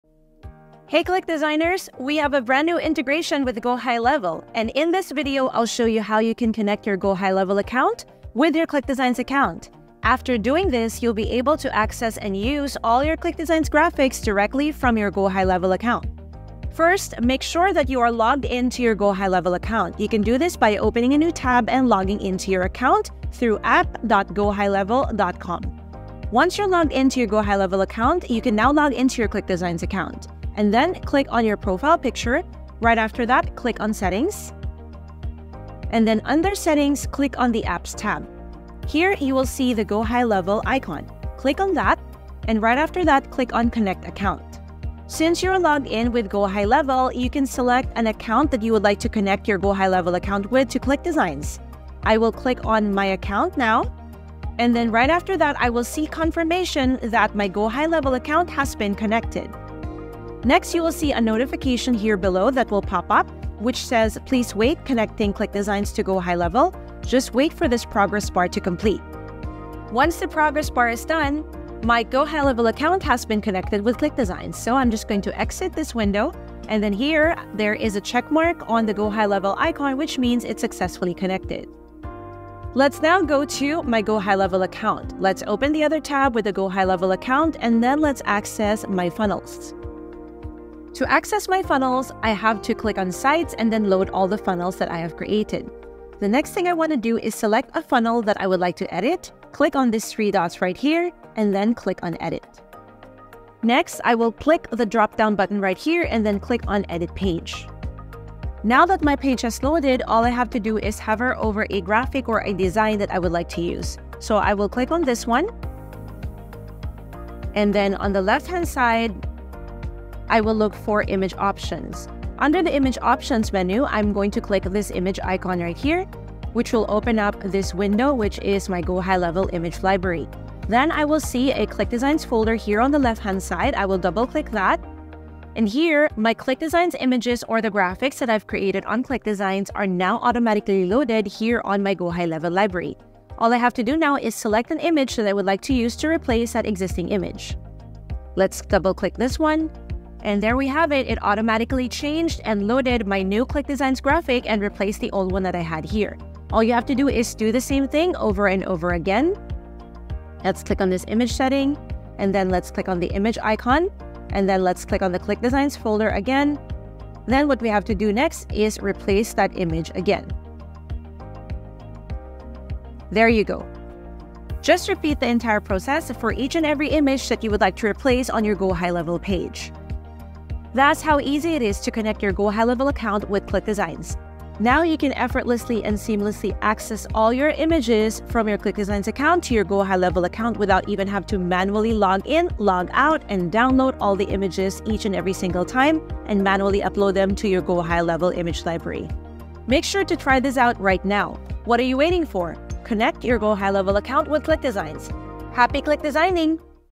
🚀 Streamline Your Website Creation: ClickDesigns & GoHighLevel Integration! 💻✨
🌟 Exciting news! 🚀 ClickDesigns has just made website creation even smoother with our new GoHighLevel integration. Discover how this integration will supercharge your design process.

💼 Say goodbye to time-consuming hassles! With the ClickDesigns and GoHighLevel integration, creating and customizing your site is a breeze. No more manual downloads or switching tabs – your designs are seamlessly available in your GoHighLevel image library. ⏰✨

💻 Want to create websites faster? This integration has you covered! Access your graphics quicker and create stunning sites without any fuss. 🌈💪

⚙️ Streamline your graphics, boost your efficiency, and watch your online presence soar like never before! 🌐📈

✅ Create Amazing Designs & Graphics For Your Blogs, Websites, Sales Funnels & Site Builders in 60 Seconds!